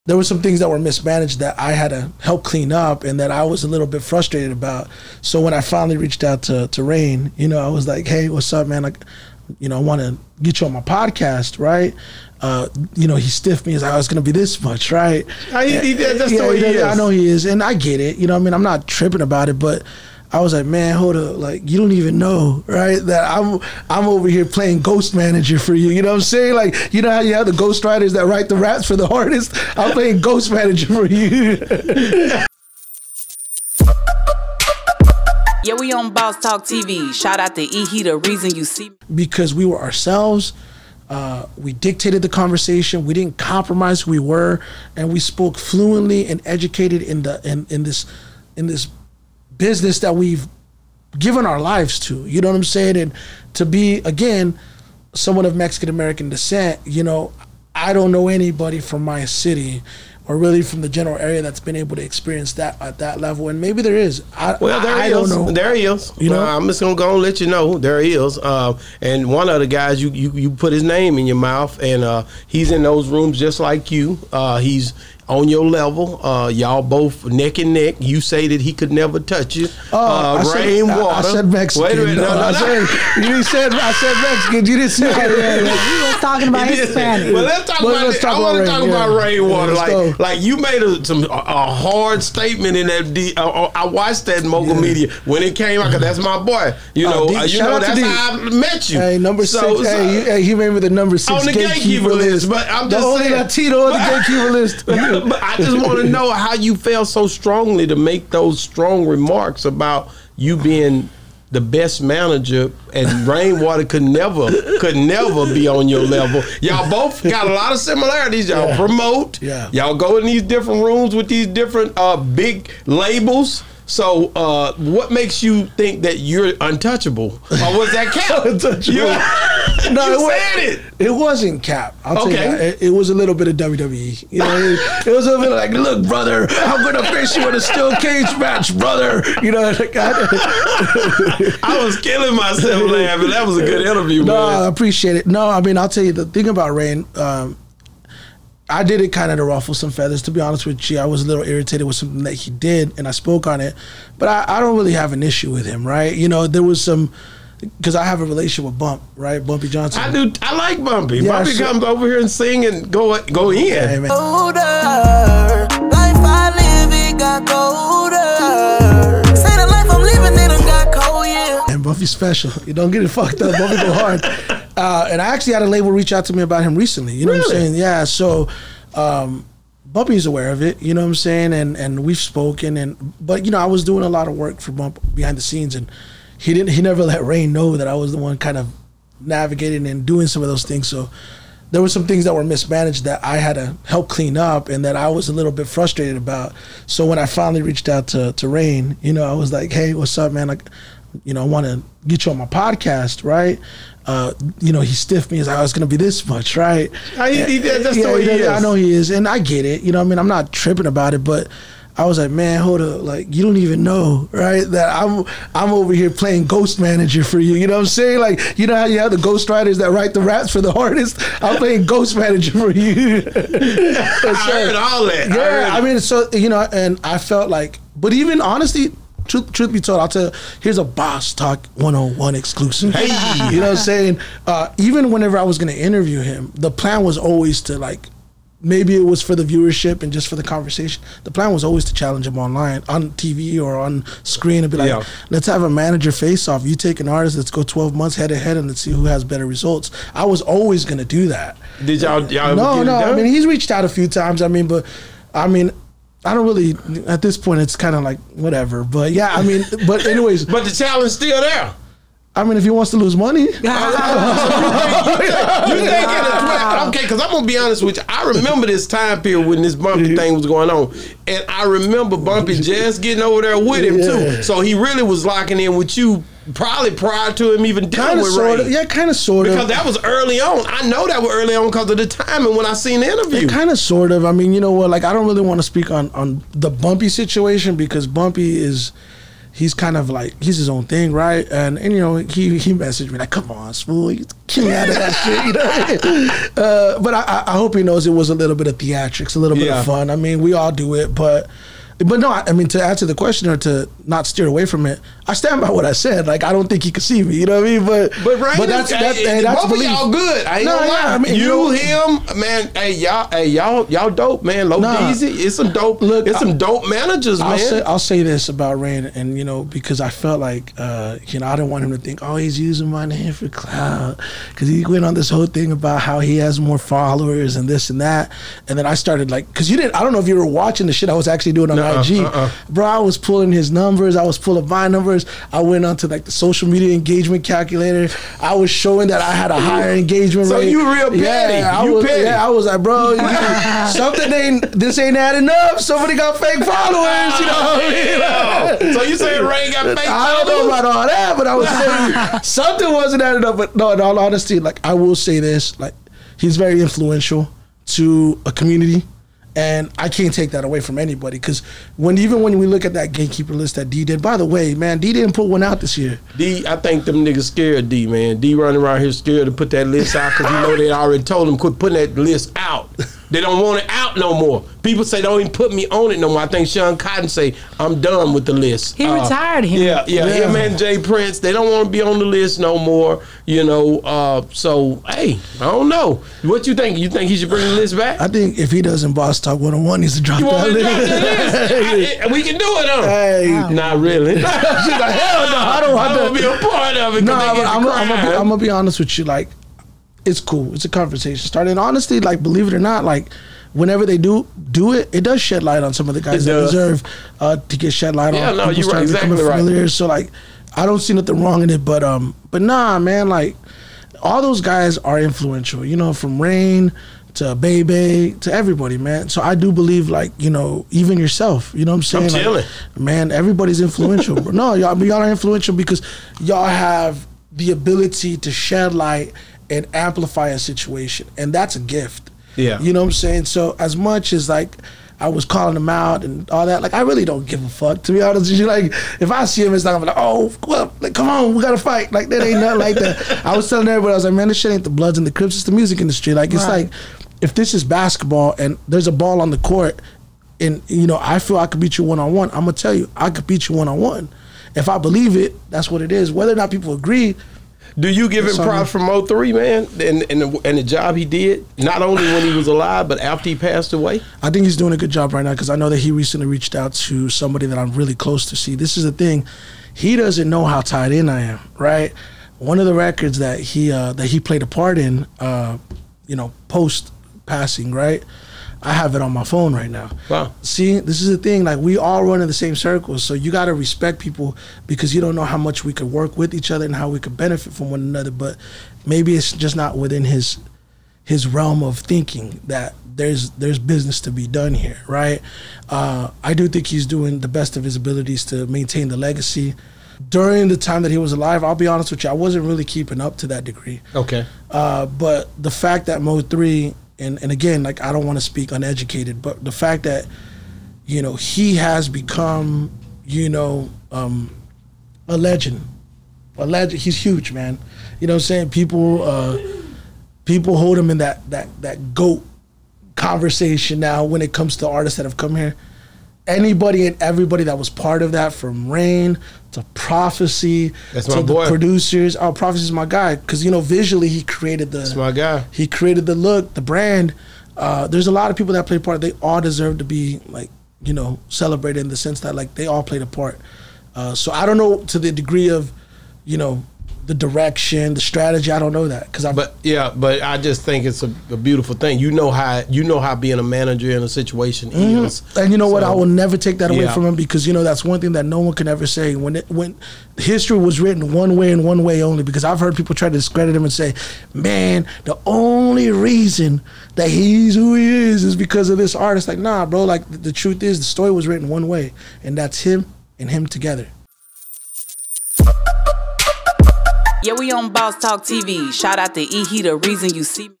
SmoothVega Call Out Rainwater You Tried to Charge Me I Ghost Managed For You | DFW MGR ( Part 1)
SmoothVega Called Out Rainwater For Trying to Charge Him Got a Interview

Executive Producer: Chasity Swilley
Producer: Stephanie Smith
Video Manager: Dennis Cooper
Content Manager: Shamariah Smith
Public Relations: Tre' Smith